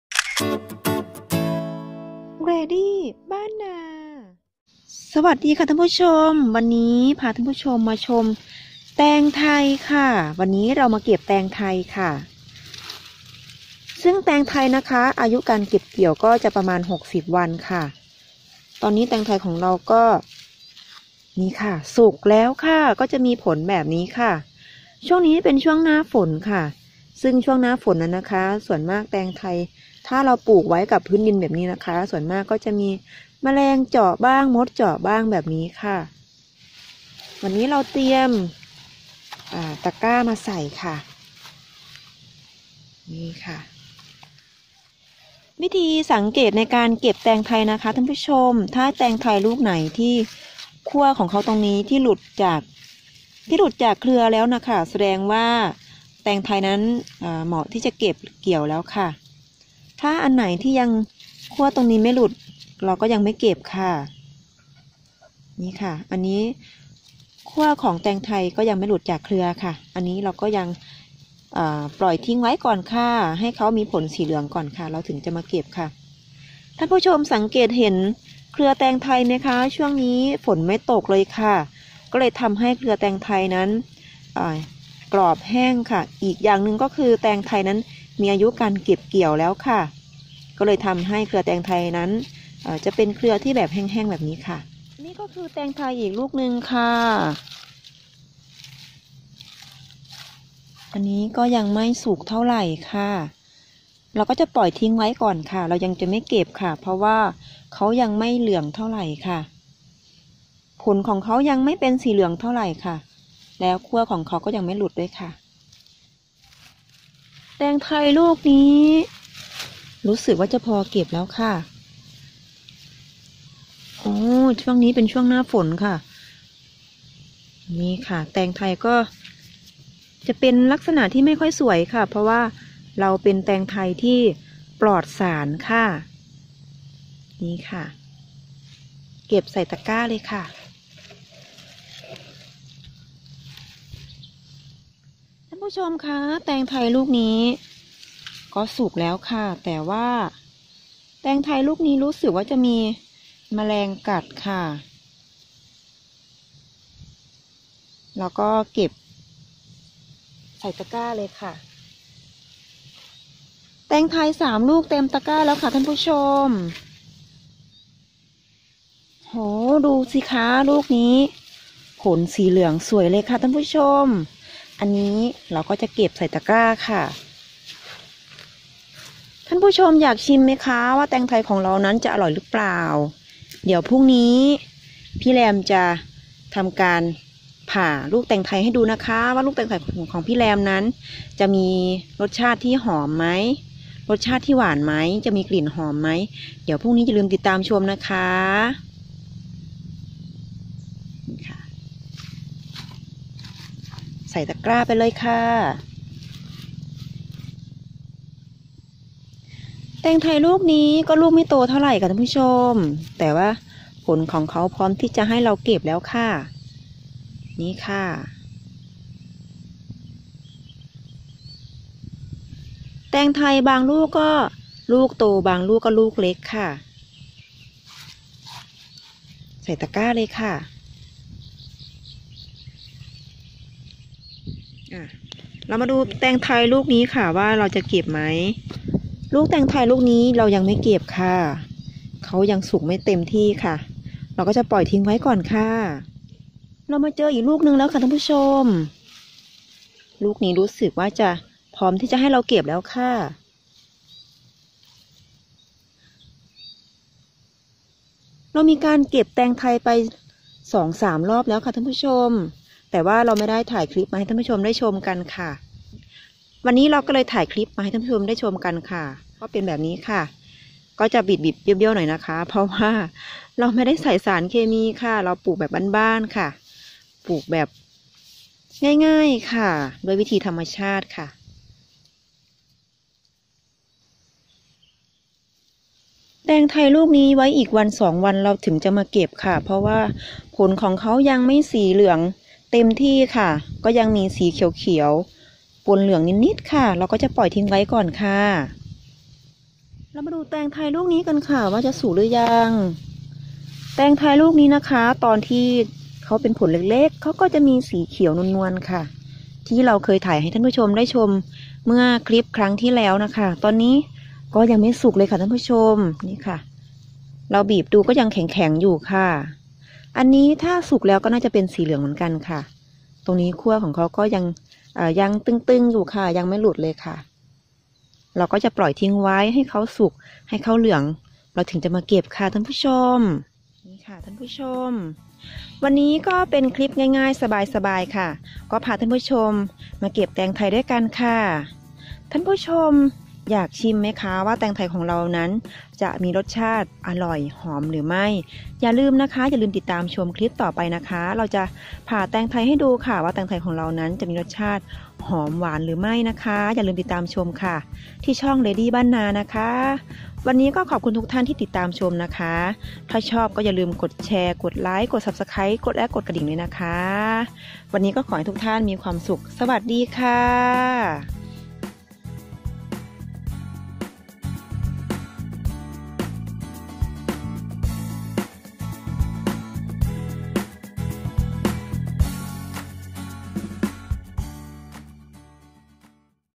เก็บแตงไทยที่สวน
สวัสดีค่ะวันนี้พาเพื่อนๆมาเก็บแตงไทยที่สวนค่ะ